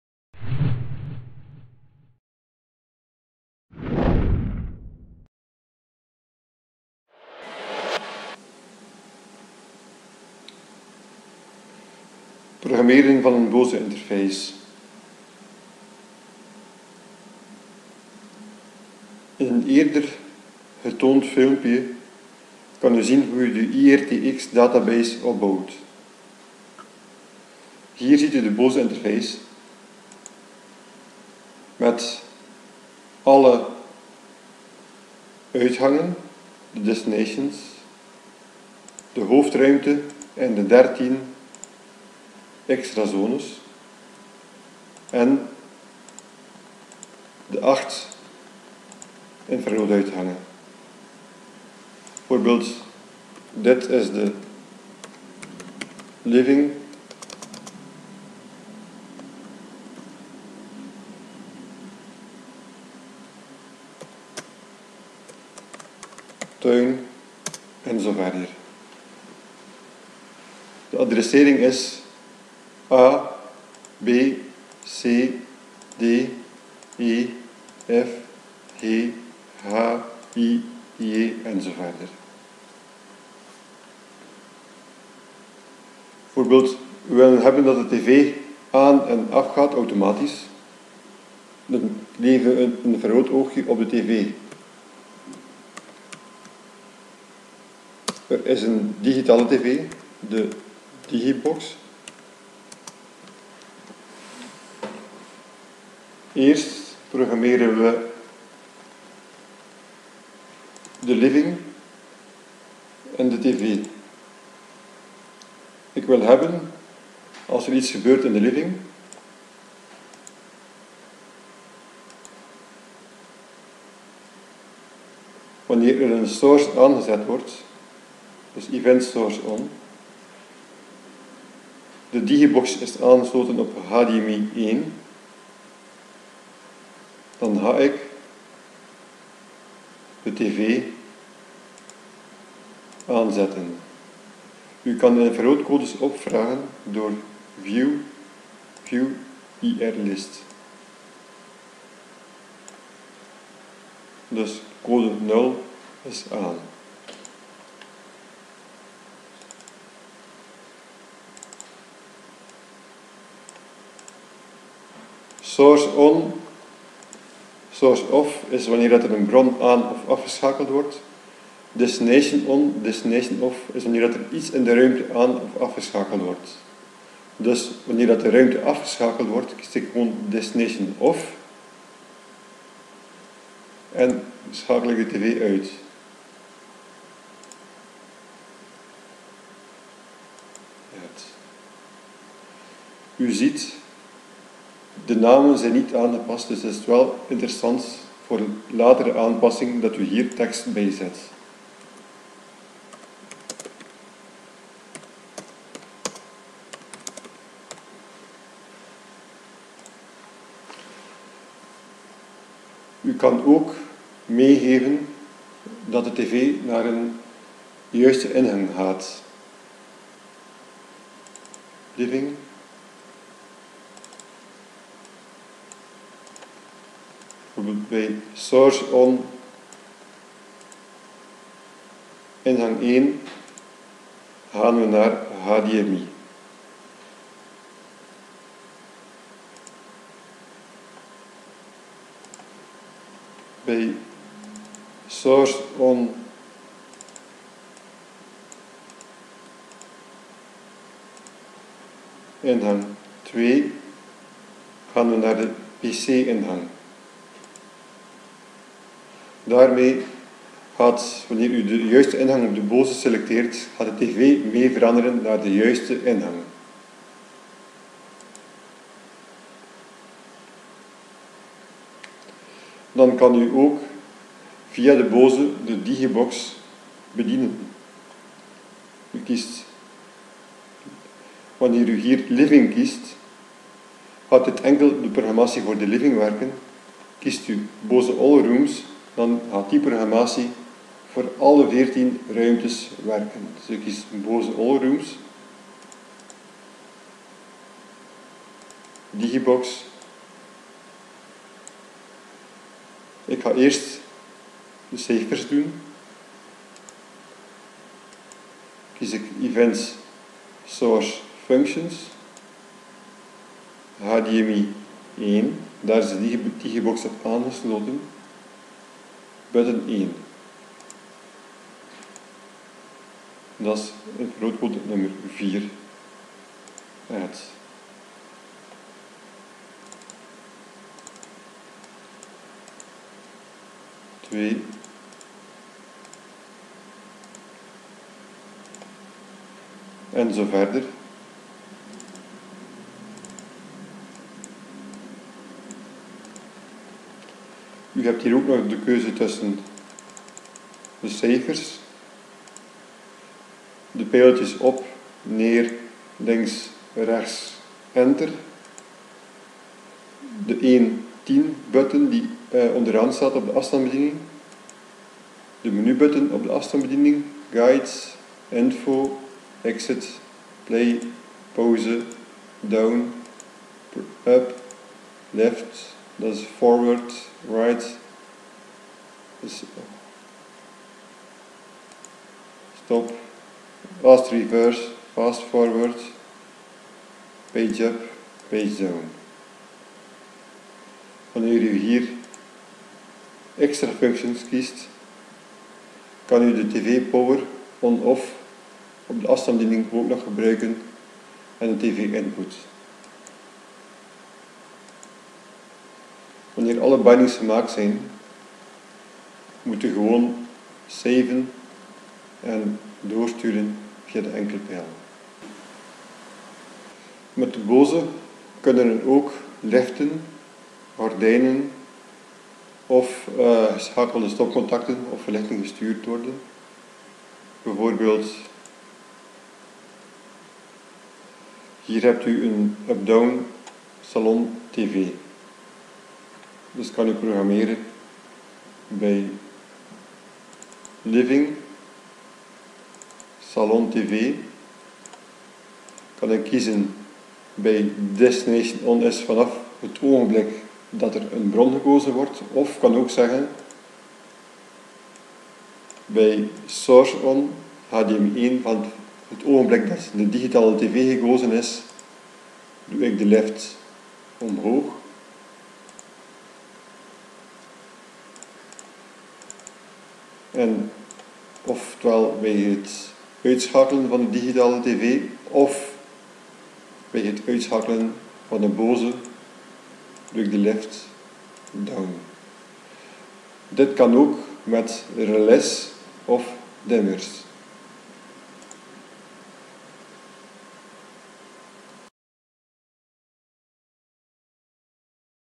Software - Programmatie Bose interface
Het configureren van de Bose interface in de Duotecno Binding Software met gebruik van de IR database.